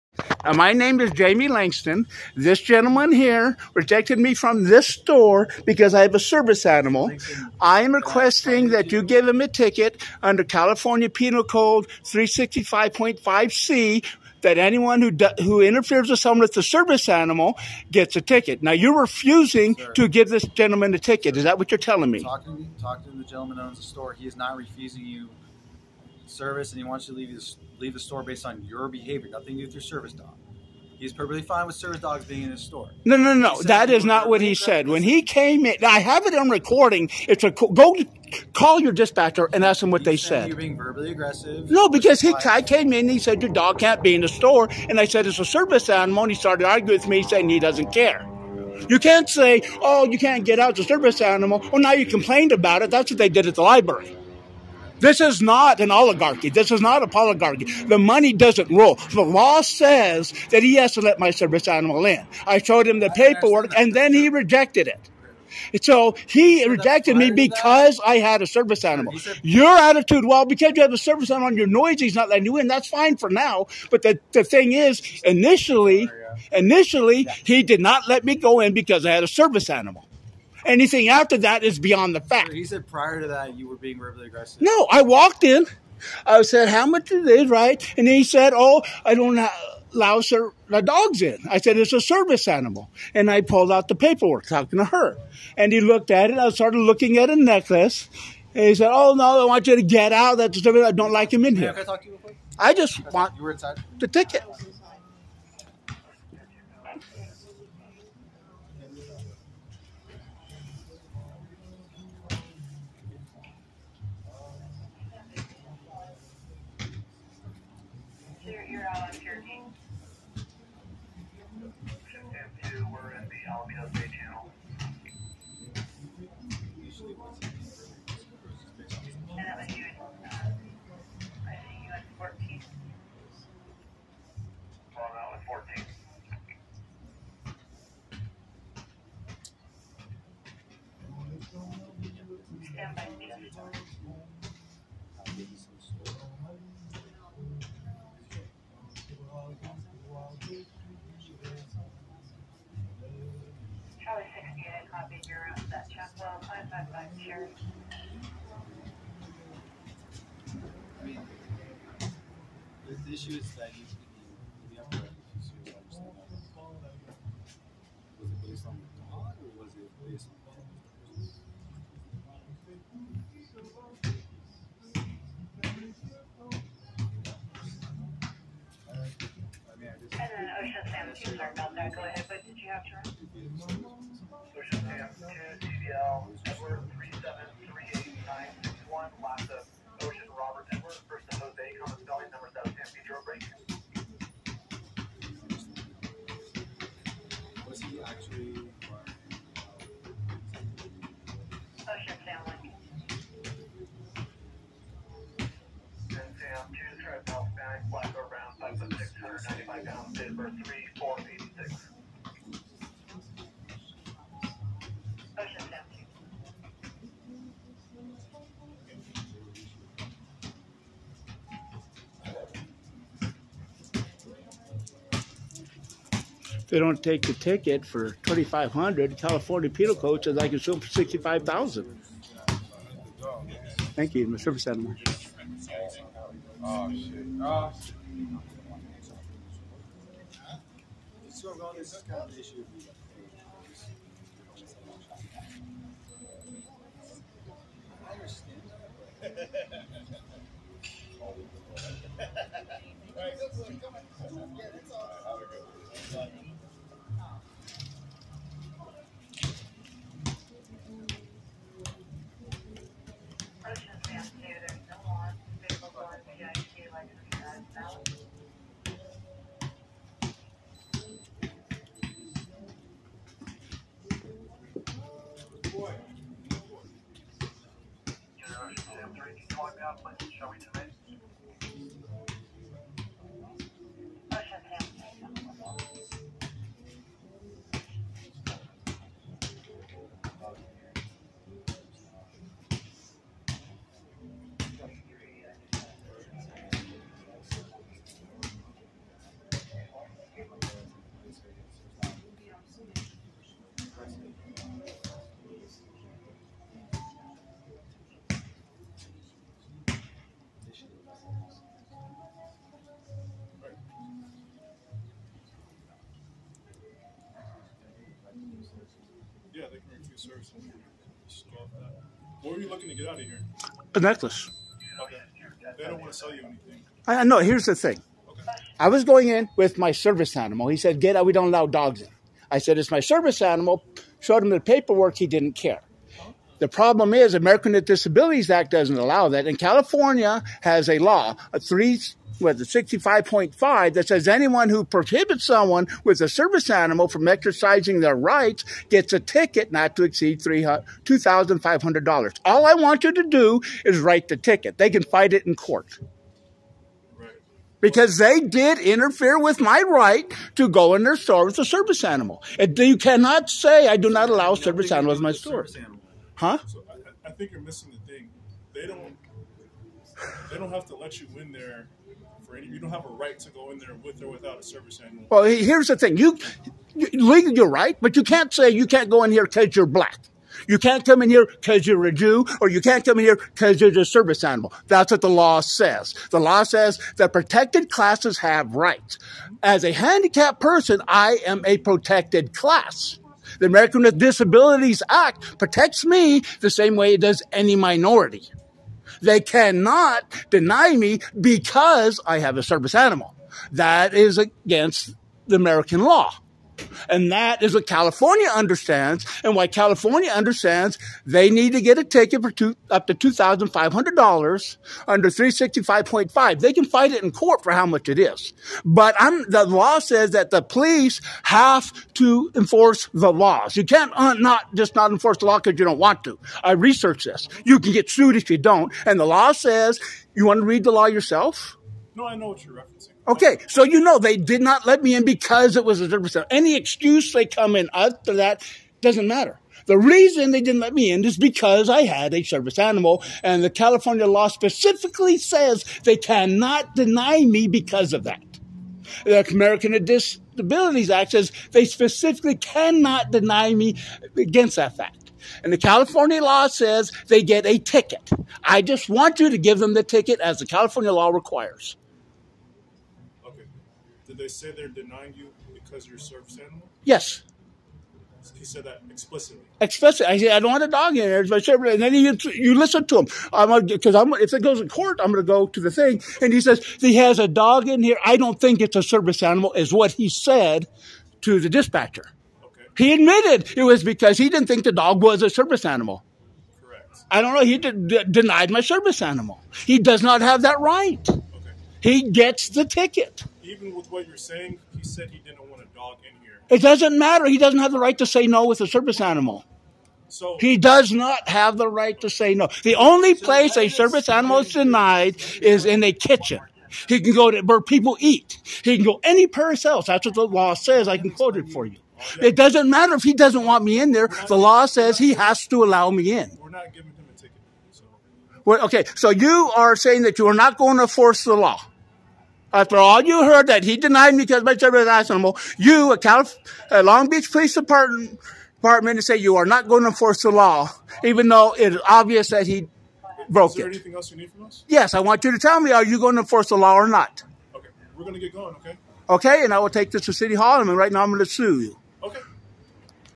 Police refuse to enforce the law
a merchant doesn't want dogs in his store, rejects my service animal, and long beach police refuse to enforce law.